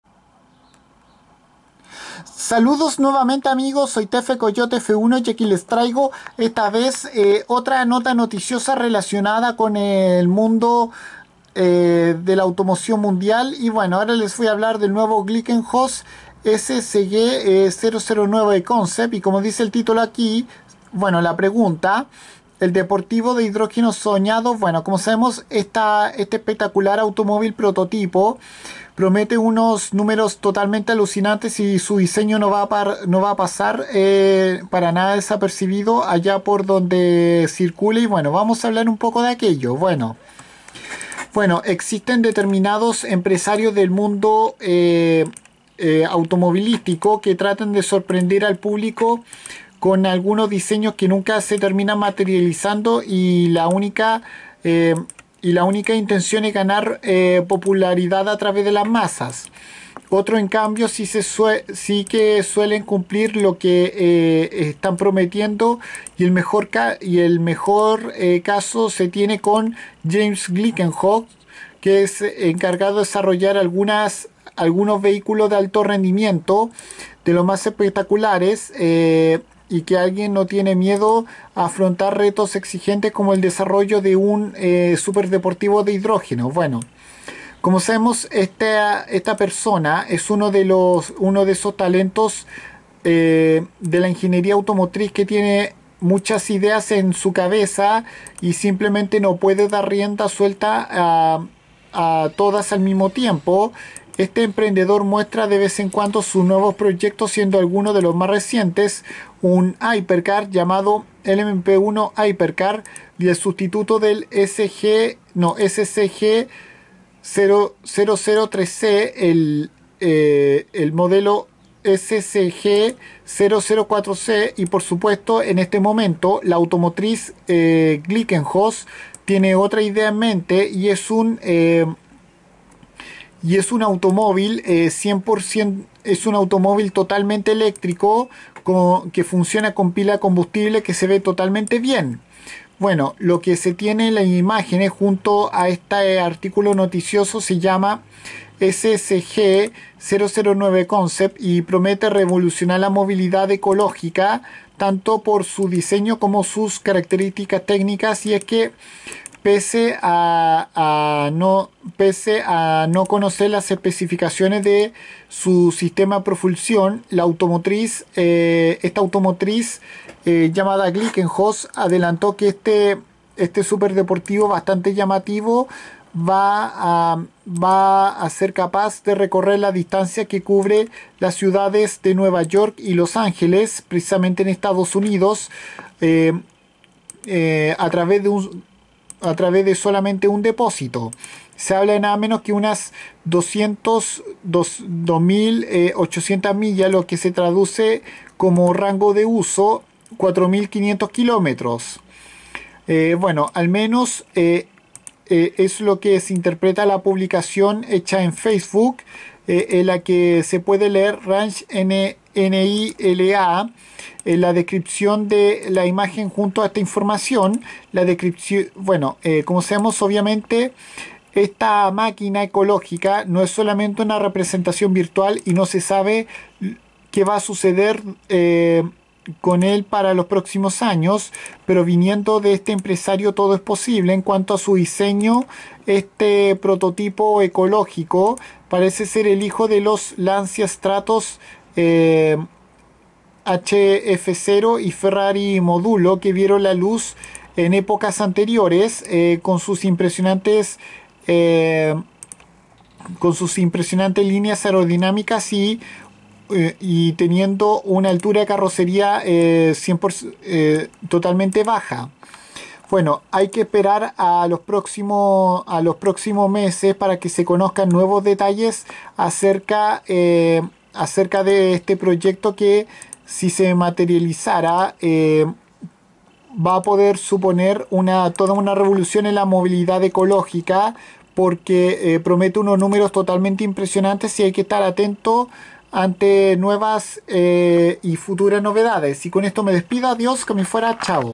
Glickenhaus SCG009 Concept información
En este vídeo les enseño sobre este automóvil de concepto totalmente eléctrico impulsado por un motor de pila de combustible de hidrógeno